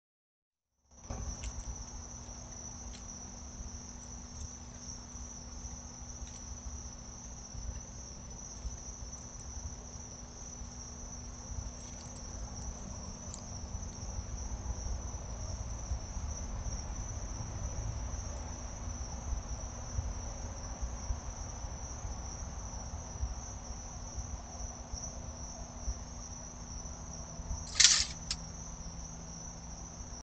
Baby Opossum caught in rat trap!
I put out a live catch trap for some rats that raid the bird feeders at night and unexpectedly caught baby opossum.  He was feed and released at night.